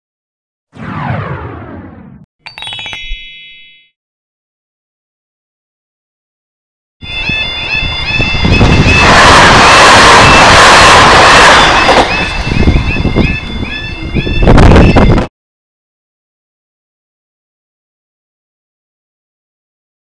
NIR Class 3000 No.3321 at Speed at Glarryford Level Crossing
An Translink Northern Ireland Railways CAF class 3000 no. 3321 at top speed / thrash at Glarryford level crossing heading for Derry/Londonderry